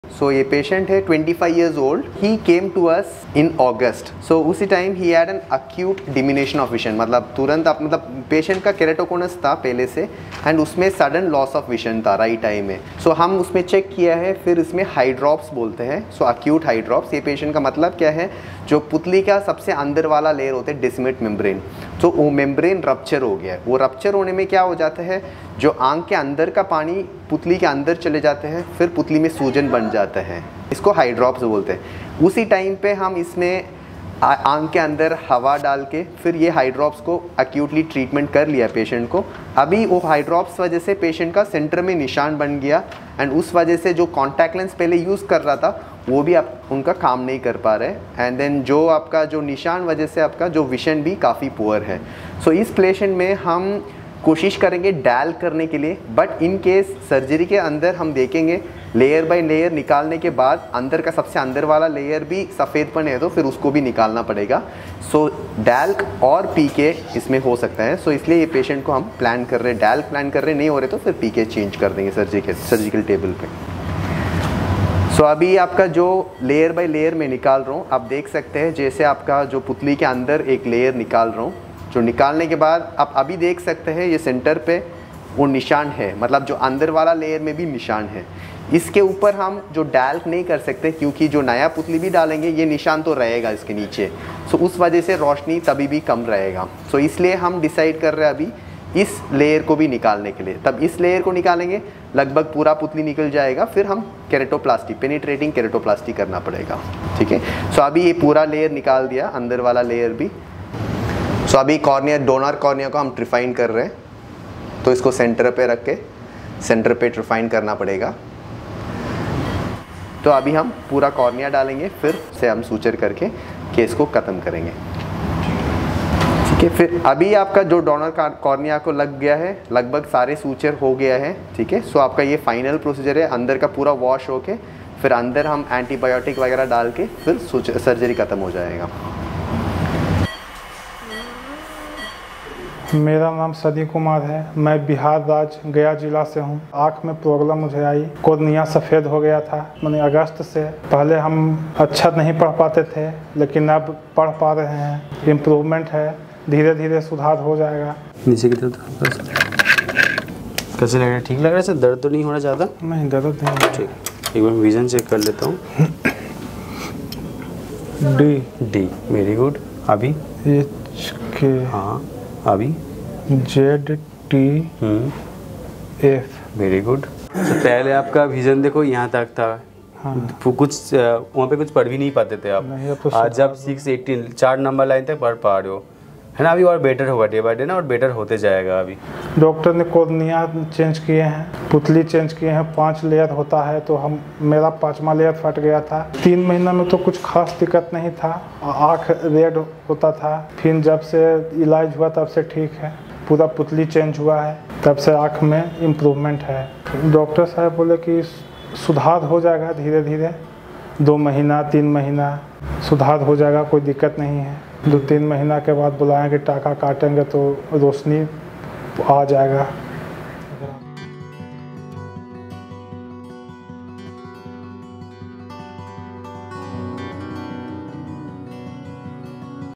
Treatment of Acute Corneal Hydrops in Keratoconus by Full-Thickness Corneal Transplant (PK)
In this video, Dr. Md Ibrahime Asif explains about an uncommon Cornea disorder named Acute Corneal Hydrops seen in a Keratoconus patient and also demonstrates its treatment by Full Thickness Corneal Transplant (Penetrating Keratoplasty)

About Acute corneal hydrops (ACH)
Acute corneal hydrops is caused by the acute disruption of Descemet's membrane in the setting of corneal ectasia. Hydrops is a term used to denote an abnormal accumulation of fluid in a body tissue or cavity.  It can cause impaired vision and eye pain. Acute corneal hydrops occurs in approximately 3% of patients with Keratoconus and presents most commonly between 20 and 40 years of age.

About Patient
Mr. Sunny Kumar (24) is from Gaya, Bihar.
He was suffering from Keratoconus in his right eye. As the cornea becomes weak due to Keratoconus, the internal layer of the cornea ruptures and aqueous fluid of the eye accumulated in the cornea and caused swelling and whiteness which impaired his vision.

He underwent full thickness corneal transplant at Eye7 Eye Hospitals, Lajpat Nagar, New Delhi.
Fortunately, his vision in his right eye has improved to 6/18 which is actually a huge success.

👨‍⚕️ABOUT THE SURGEON - DR. MD IBRAHIME ASIF
(Cornea Specialist at Eye7 Eye Hospitals, Lajpat Nagar previously worked at AIIMS-Delhi)

📽️ MORE VIDEOS ON VARIOUS TYPES OF CORNEAL TRANSPLANTS